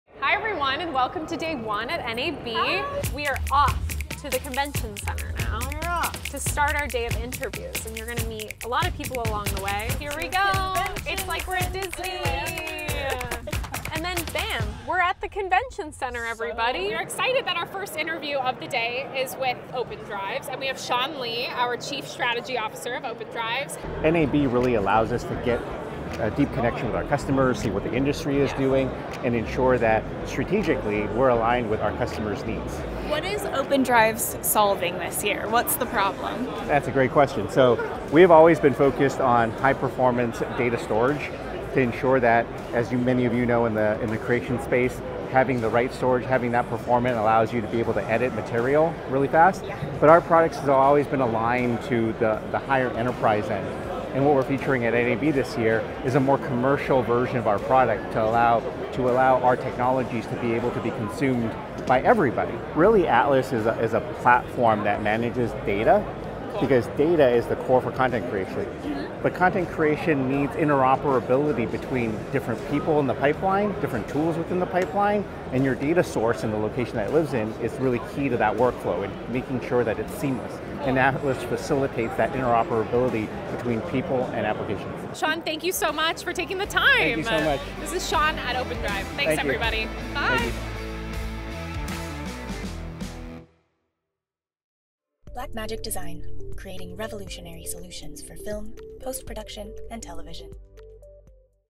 NAB 2024: Cinematography for Actors Chats With Open Drives
Get the latest in storage solutions from Open Drives from our No Film School x CFA collab direct from the convention center floor at NAB 2024.